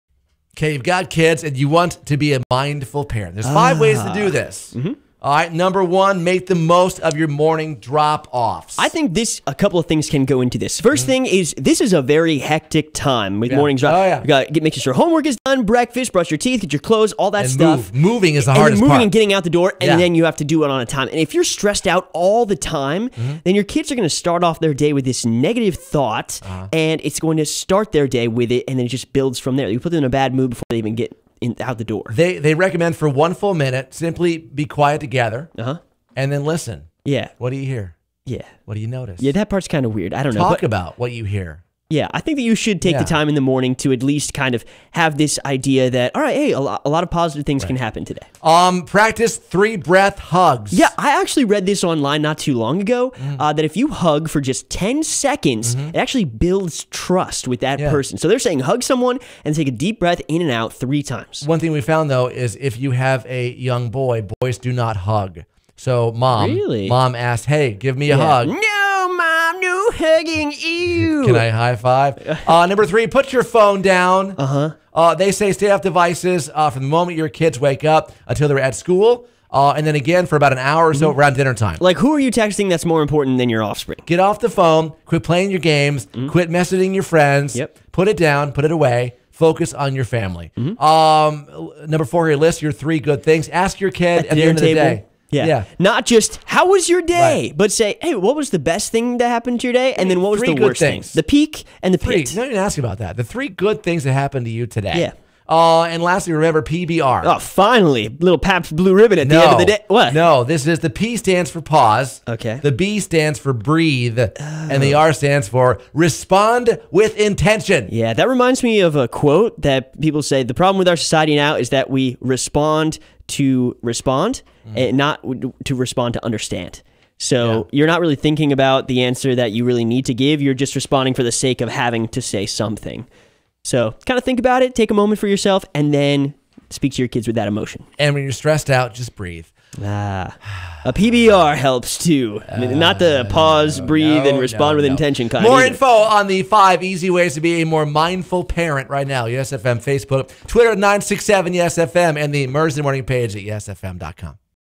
5 Easy Ways to Be a More Mindful Parent
Mindfulness has been a buzzy wellness concept for years now, with evidence touting its potential impact on reducing stress and improving focus.
Then there’s mindful parenting, which is kind of its own beast. By being present with your kids, the thinking goes, and slowing things down for just a bit, parents and kids are able to connect in more meaningful ways — and maybe eliminate stress for everyone.